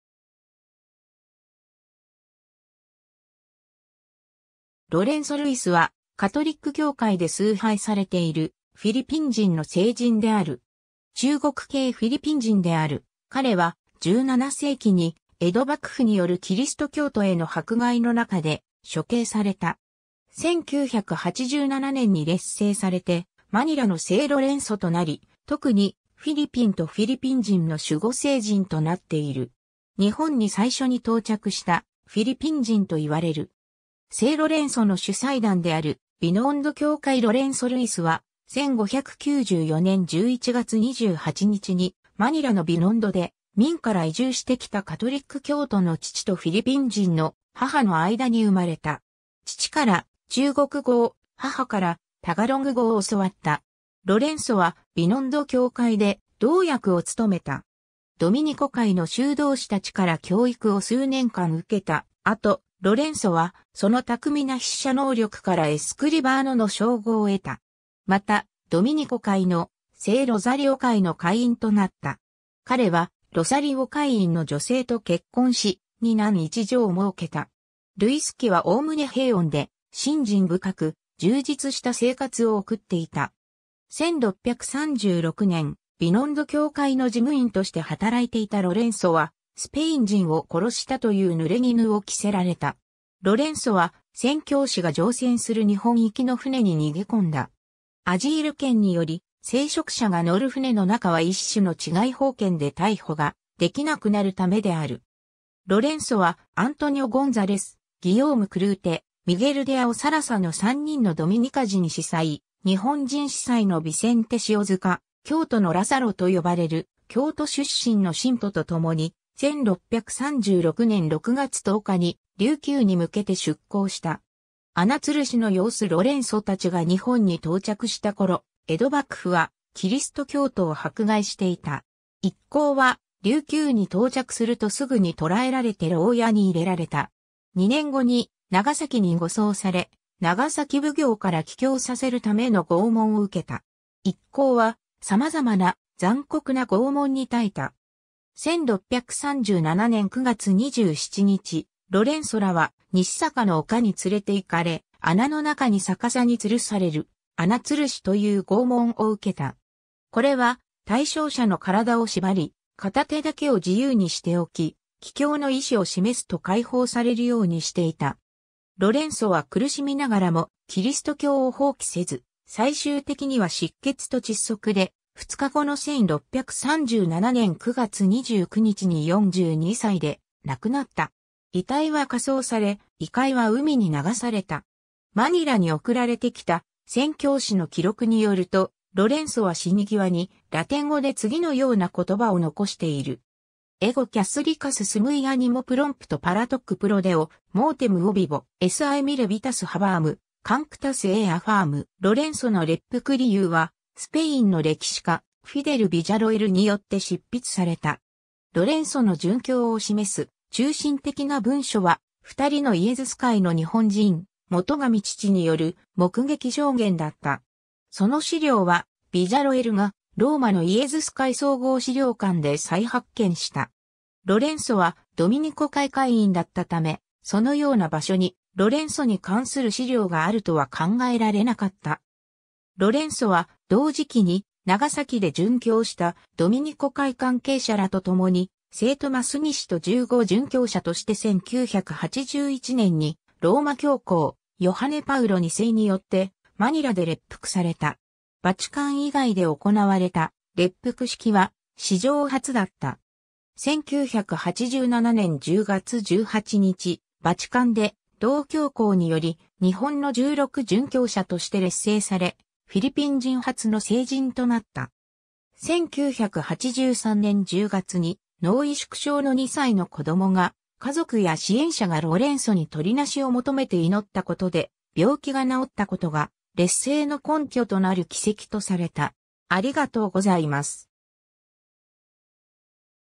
ロレンソ・ルイス
ロレンソ・ルイス（Lorenzo Ruiz、1594年11月28日 - 1637年9月29日）は、カトリック教会で崇拝されているフィリピン人の聖人である。
中国系フィリピン人である彼は、17世紀に江戸幕府によるキリスト教徒への迫害の中で処刑された。
1987年に列聖されてマニラの聖ロレンソとなり、特にフィリピンとフィリピン人の守護聖人となっている。
日本に最初に到着したフィリピン人といわれる。
 聖ロレンソの主祭壇であるビノンド教会 ロレンソ・ルイスは、1594年11月28日にマニラのビノンドで、明から移住してきたカトリック教徒の父とフィリピン人の母の間に生まれた。
父から中国語を、母からタガログ語を教わった。
 ロレンソは、ビノンド教会で堂役を務めた。
ドミニコ会の修道士たちから教育を数年間受けた後、ロレンソは、その巧みな筆写能力からエスクリバーノ（書士）の称号を得た。
また、ドミニコ会の聖ロザリオ会の会員となった。
彼はロサリオ会員の女性と結婚し、2男1女をもうけた。
ルイス家はおおむね平穏で、信心深く、充実した生活を送っていた。
 1636年、ビノンド教会の事務員として働いていたロレンソは、スペイン人を殺したという濡れ衣を着せられた。
ロレンソは、宣教師が乗船する日本行きの船に逃げ込んだ。
アジール権により、聖職者が乗る船の中は一種の治外法権で逮捕ができなくなるためである。
ロレンソは、アントニオ・ゴンザレス、ギヨーム・クルーテ、ミゲル・デ・アオサラサの3人のドミニカ人司祭、日本人司祭のビセンテ塩塚、「京都のラサロ」と呼ばれる京都出身の信徒とともに、1636年6月10日に琉球に向けて出航した。
 「穴吊るし」の様子 ロレンソたちが日本に到着した頃、江戸幕府はキリスト教徒を迫害していた。
一行は琉球（現在の沖縄県）に到着するとすぐに捕らえられて牢屋に入れられた。
2年後に長崎に護送され、長崎奉行から棄教させるための拷問を受けた。
一行は様々な残酷な拷問に耐えた。
 1637年9月27日、ロレンソらは西坂の丘に連れて行かれ、穴の中に逆さに吊るされる「穴吊るし」（「吊り殺し」、または単に「吊るし」ともいう）という拷問を受けた。
これは、対象者の体を縛り、片手だけを自由にしておき、棄教の意思を示すと解放されるようにしていた。
ロレンソは苦しみながらもキリスト教を放棄せず、最終的には失血と窒息で、2日後の1637年9月29日に42歳で亡くなった。
遺体は火葬され、遺灰は海に流された。
 マニラに送られてきた宣教師の記録によると、ロレンソは死に際にラテン語で次のような言葉を残している。
 Ego Catholicus sum et animo prompto paratoque pro Deo mortem obibo. Si mille vitas haberem, cunctas ei offerrem. （私はカトリック教徒であり、神のために死を受け入れることを心の底から望んでいます。 もし私が千の命を持っていたら、これら全てを神に捧げましょう。） ロレンソの列福理由（Positio Super Introductione Causae）は、スペインの歴史家フィデル・ビジャロエルによって執筆された。
ロレンソの殉教を示す中心的な文書は、2人のイエズス会の日本人元神父による目撃証言だった。
その資料は、ビジャロエルがローマのイエズス会総合資料館で再発見した。
ロレンソはドミニコ会会員だったため、そのような場所にロレンソに関する資料があるとは考えられなかった。
 ロレンソは、同時期に長崎で殉教したドミニコ会関係者らとともに、「聖トマス西と15殉教者」として1981年にローマ教皇ヨハネ・パウロ2世によってマニラで列福された。
バチカン以外で行われた列福式は史上初だった。
1987年10月18日、バチカンで同教皇により「日本の16殉教者」として列聖され、フィリピン人初の聖人となった。
 1983年10月に脳萎縮症（水頭症）の2歳の子供が、家族や支援者がロレンソに執り成しを求めて祈ったことで病気が治ったことが、列聖の根拠となる奇跡とされた。